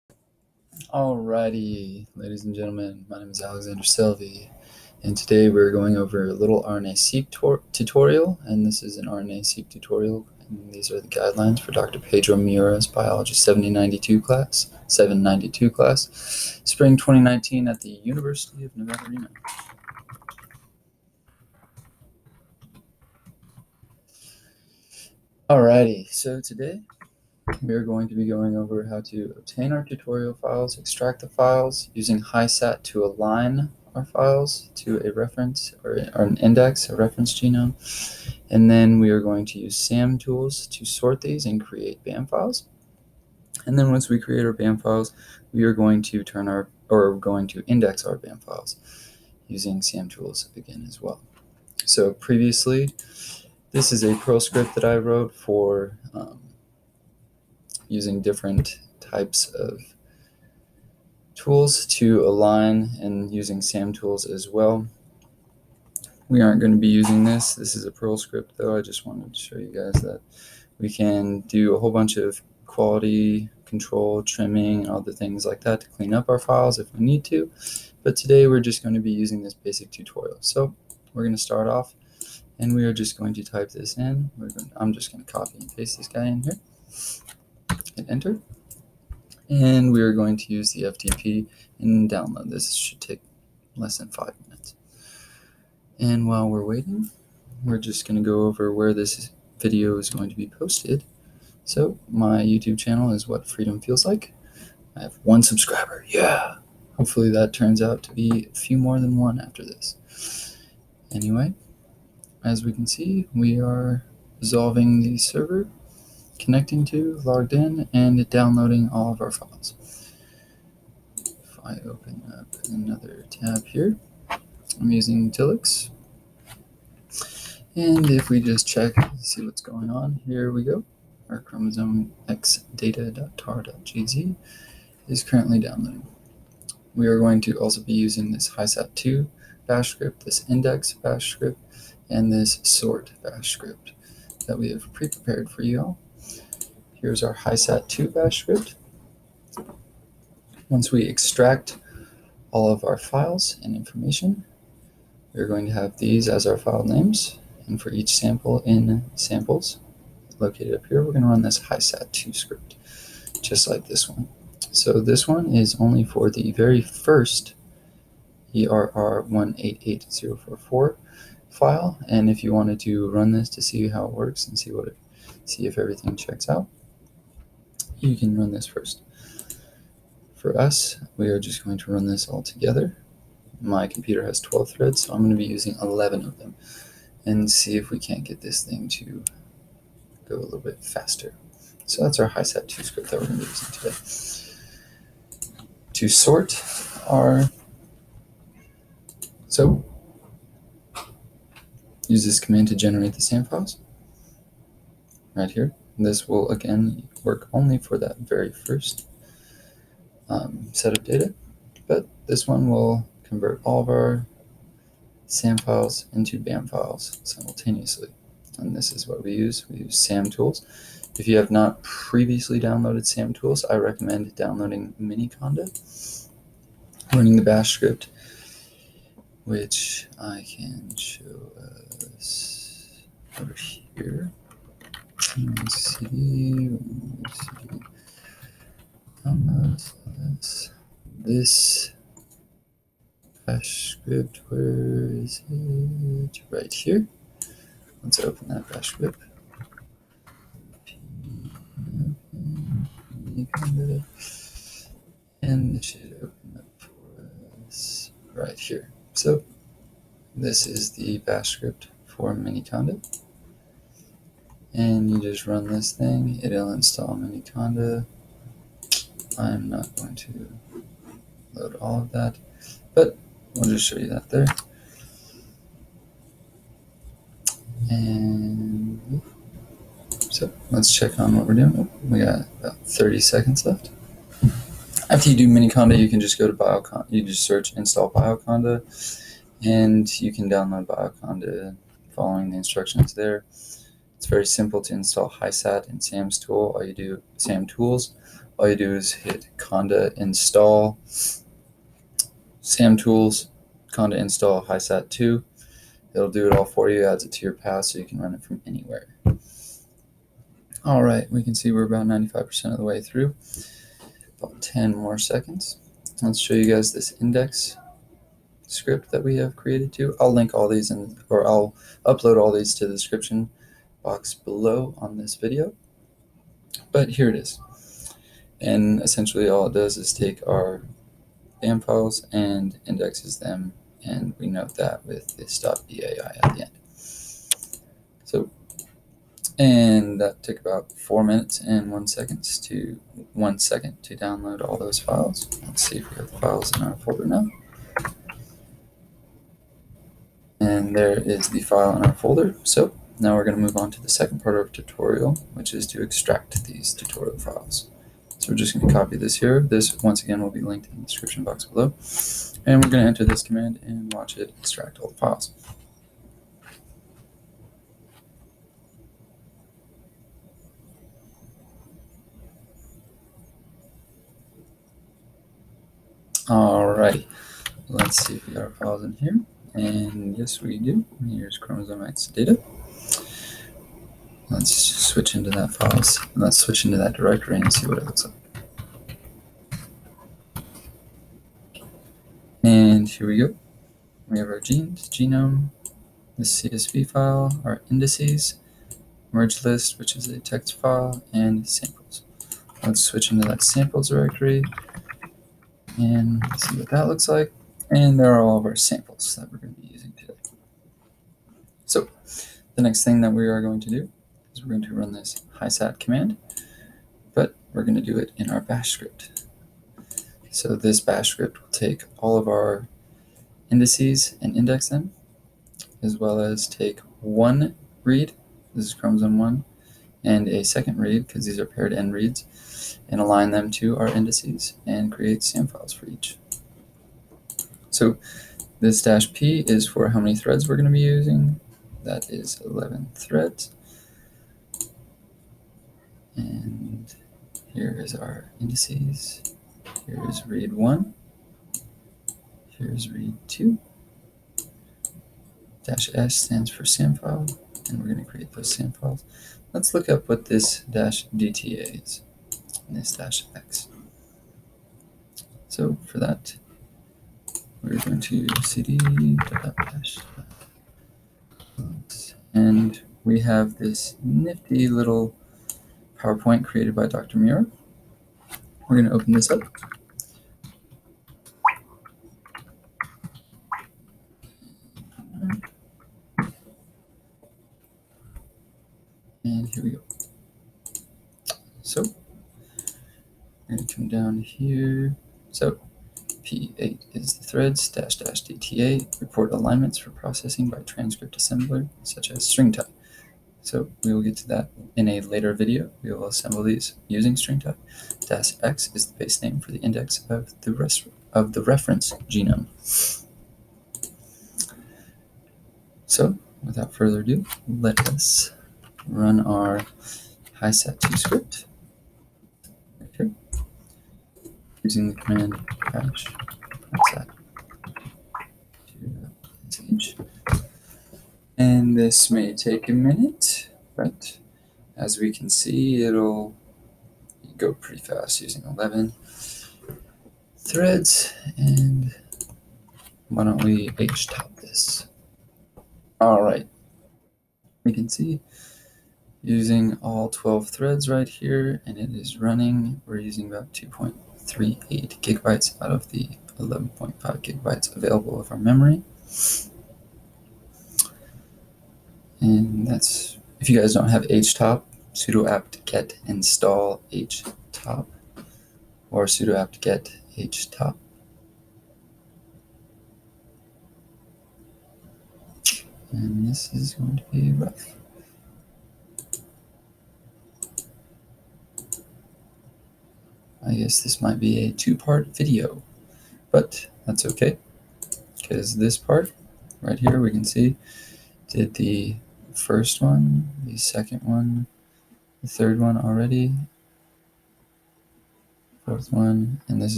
Beginner's RNA Seq Tutorial Part 1 Dr Pedro Miura's BIOL792 Course, University of Nevada, Reno
Beginners tutorial using pre-prepared rna-seq data. Alignment done with Hisat2, .bam files generated using samtools, and indexed .bam.bai files generated using samtools.

For a full workup of the code in this part of the protocol, check out my .Rmd on RPubs